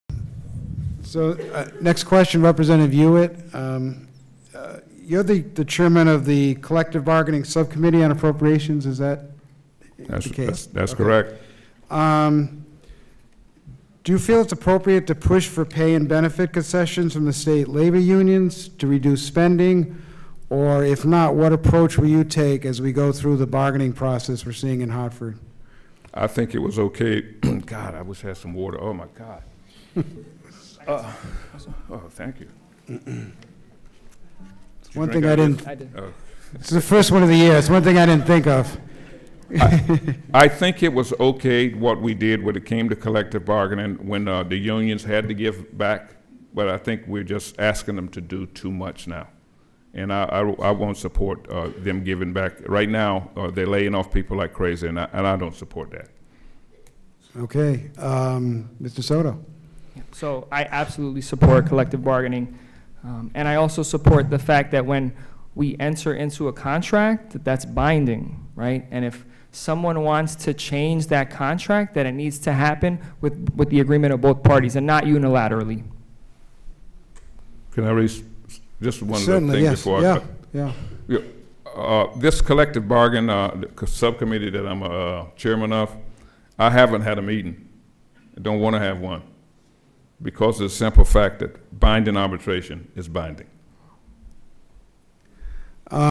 11. Collective bargaining and union concessions
New London 39th House District Democratic Primary Debate: Rep. Ernest Hewett and Chris Soto